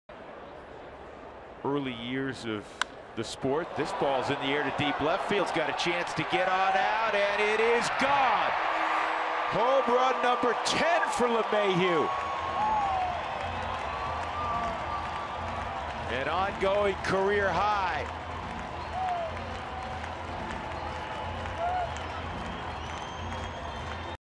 CHC@COL: LeMahieu launches a solo homer to left field
DJ LeMahieu extends the Rockies' lead to 11-2 with a solo home run to left field in the bottom of the 8th inning